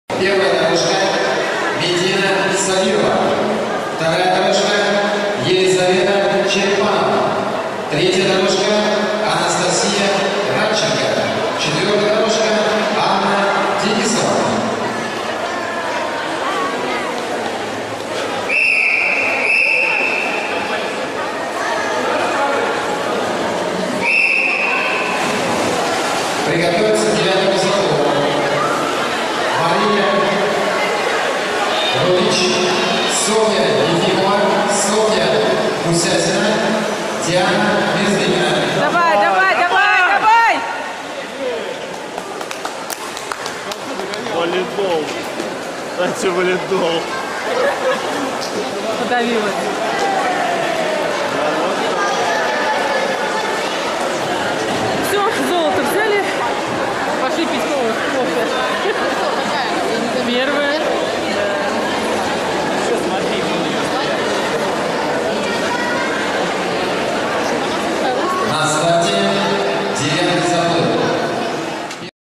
finswimming, 11,12.2011, Maardu, Estonia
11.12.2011 Закрытый чемпионат г.Маарду по плаванию в ластах
25 м ракетой